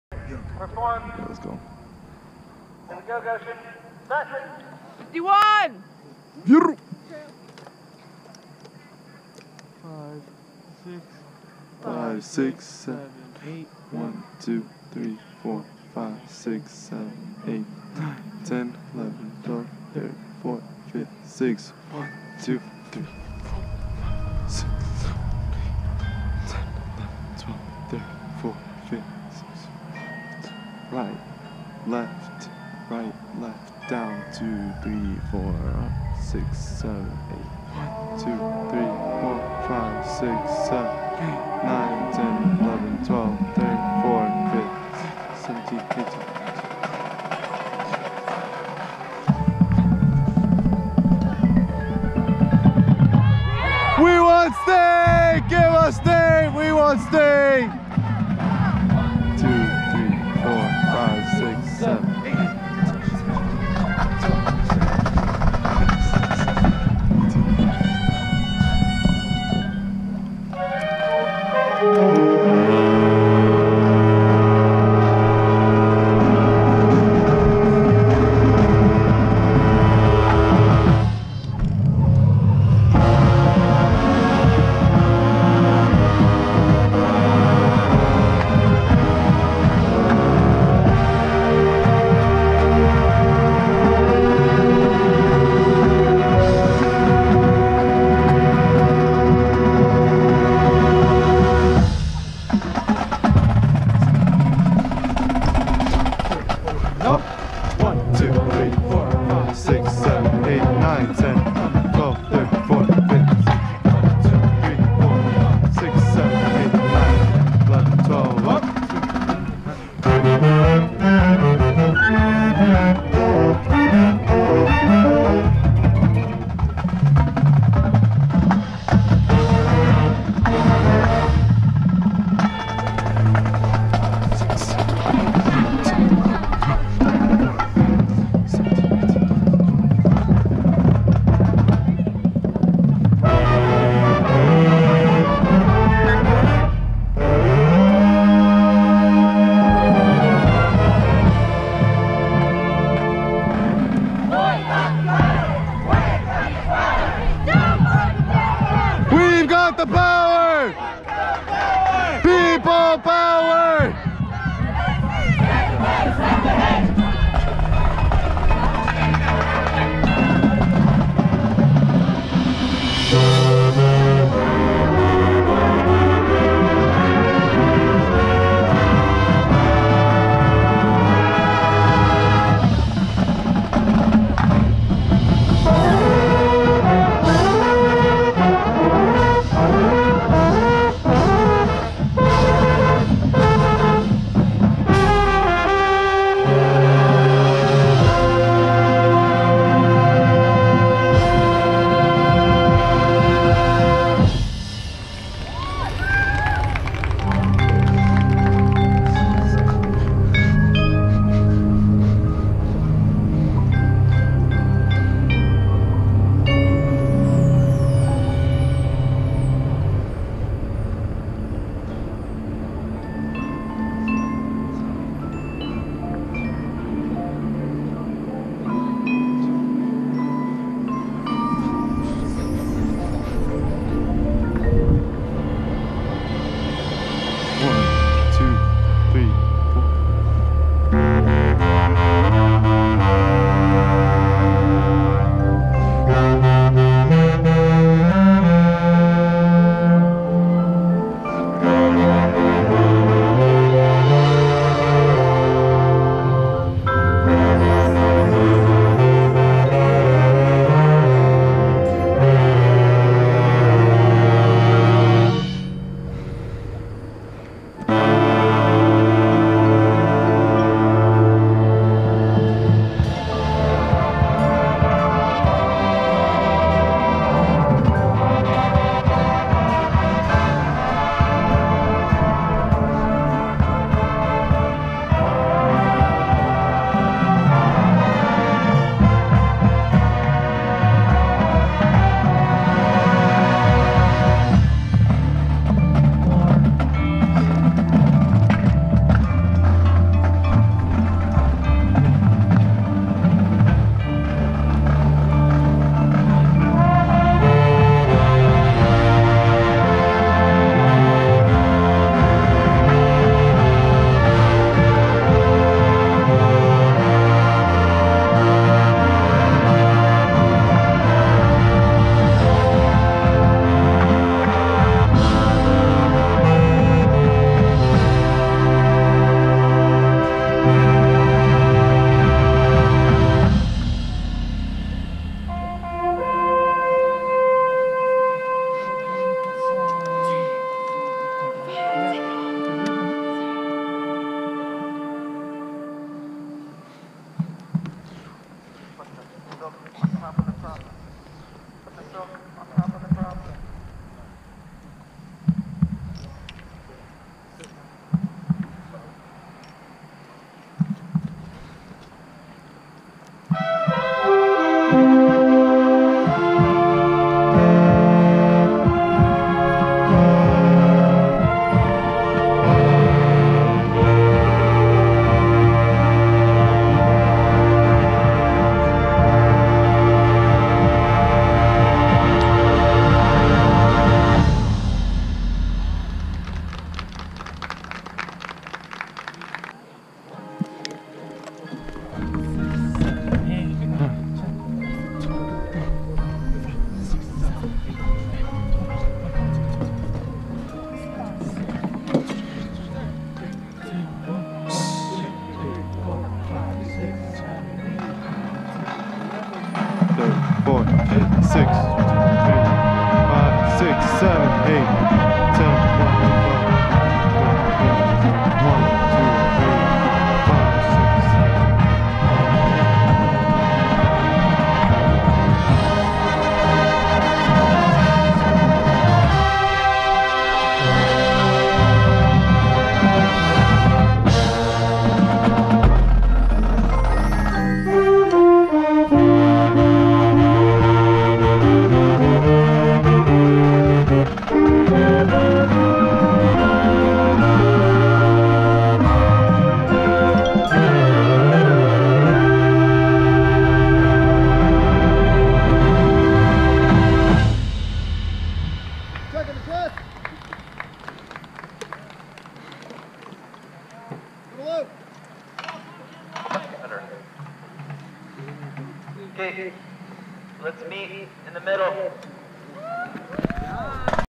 Goshen Crimson Marching Band 9/19/19 - Bass Clarinet Cam
Player: Roman Zamora
Show: Common Ground

Here we see Roman and how he plays and what his part in the show is.